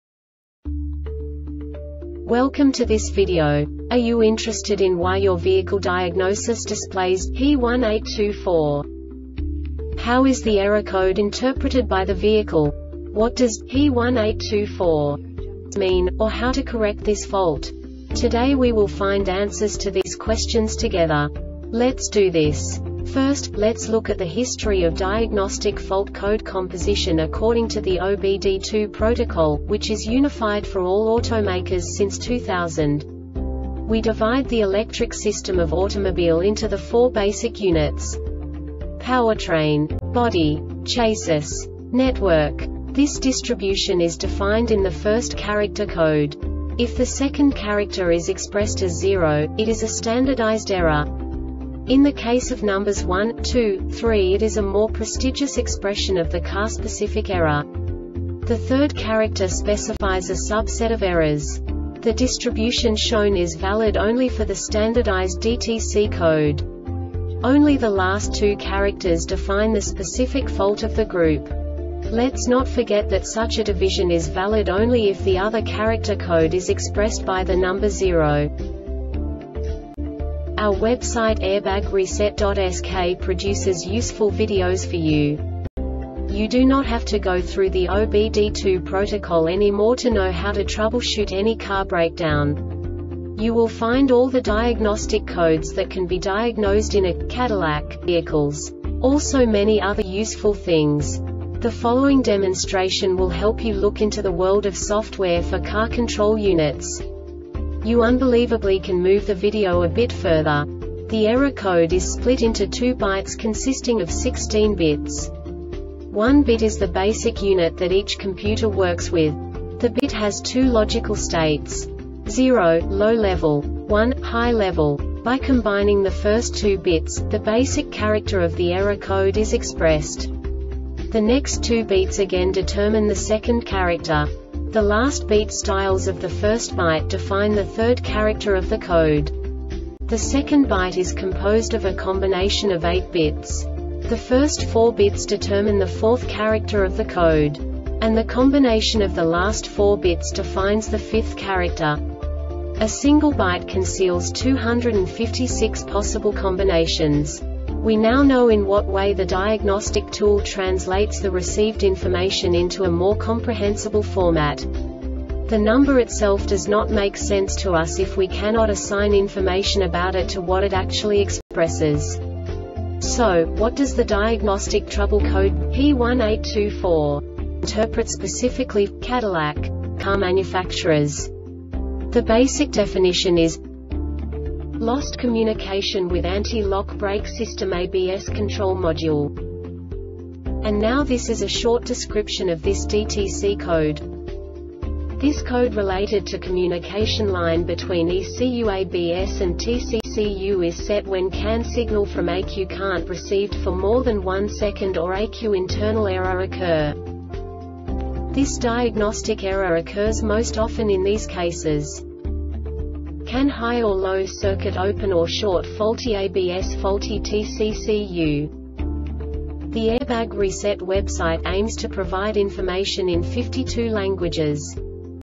DTC cadillac P1824 Short Explanation
Contents:
0:21 Basic DTC analysis according to OBD2 protocol standard.
1:48 Insight into programming
2:58 A diagnostically understandable description of the Diagnostic Trouble Code
3:05 Basic error definition

Make:
Cadillac
Code:
P1824
Definition:
Description:
This code related to communication line between ECU(ABS) and TCCU is set when CAN signal from ECU can’t received for more than 1sec or ECU internal error occur.
Cause:
 CAN HIGH or LOW circuit open or short. Faulty ABS. Faulty TCCU .